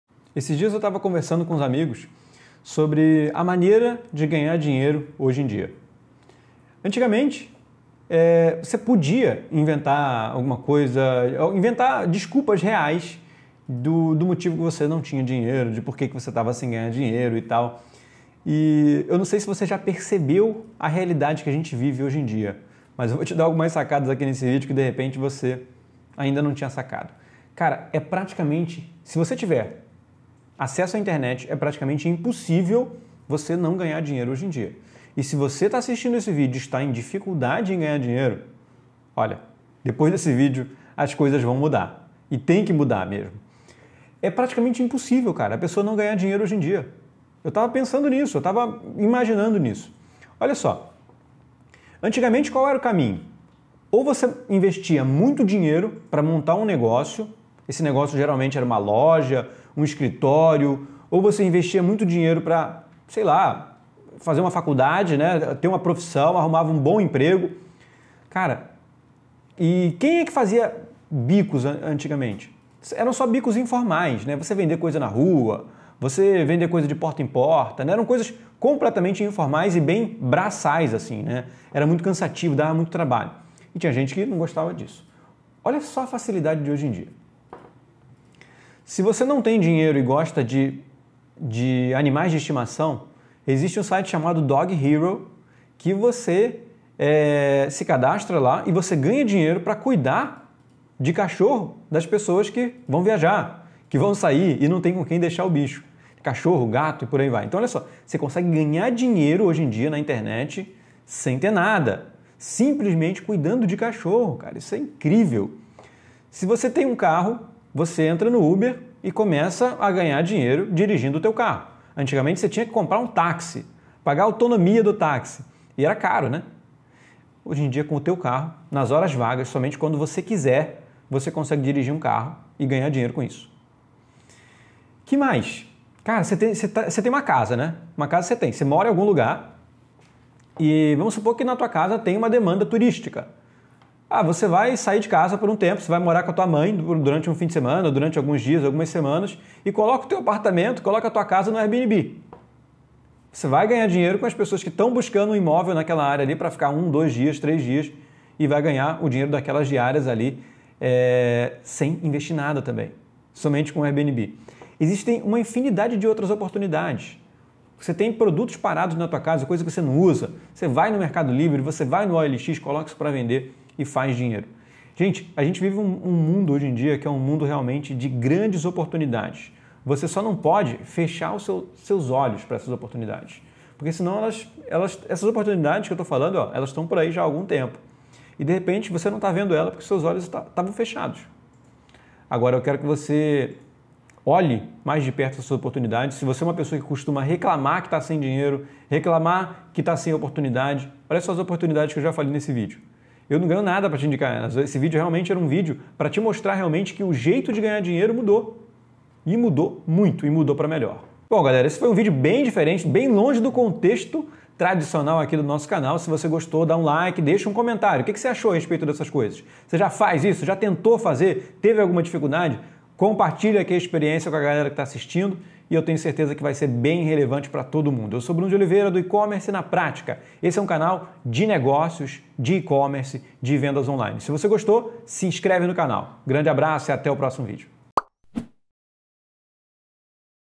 O Jeito de ganhar dinheiro mudou | Facilidades da Internet
No vídeo de hoje falaremos um pouco sobre como fazer uma renda extra na internet. Com a chegada de notebooks e smartphones às mãos de cada vez mais gente ao redor do globo, surgem infinitas possibilidades para quem quer ganhar dinheiro também em seu tempo livre (e sem precisar sair de casa).

Seja através de um pequeno negócio ou fazendo uso de serviços como Uber e Airbnb, nunca antes o homem teve tantas opções para gerar aquele desejado dinheiro extra no fim do mês.

Quer saber mais sobre o assunto? Então dê o play e vamos falar sobre algumas das suas melhores alternativas! =)

Receba o Melhor Conteúdo Exclusivo, repleto de Dicas e Estratégias de Ecommerce, Negócios e Empreendedorismo.